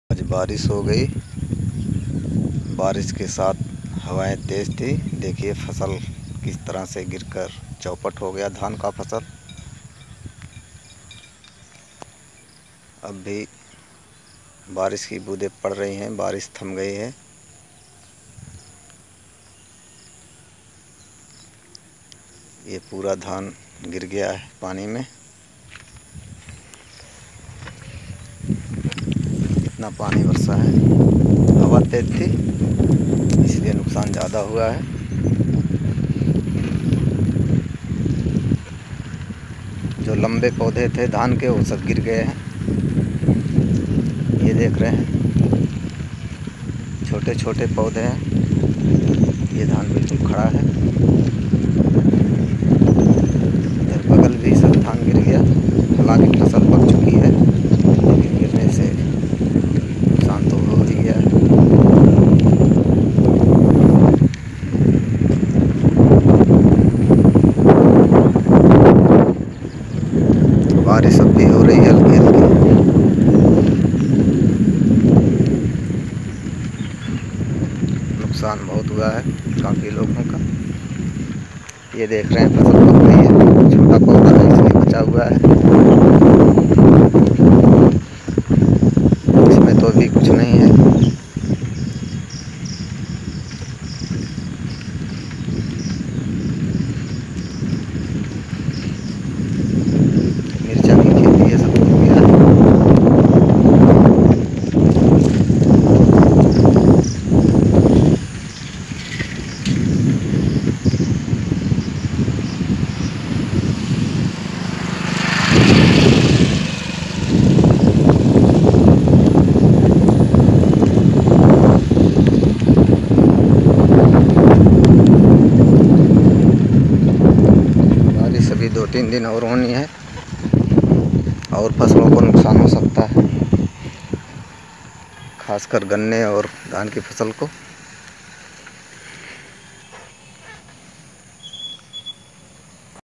Barish se faslon ki tabahi| बारिश से फसलों को हुआ भारी नुकसान
11 अक्टूबर को बारिश और तेज हवाओं की वजह से फसलों को काफी नुकसान पहुंचा। इस वीडियो में धान का फसल जो पूरी तरह से गिर चुका है उसको दिखाया गया है।

Barish ke karan nuksan hue dhaan ki fasal ka video
Barish ke Karan fasal Barbad
Barish se faslon ko nuksan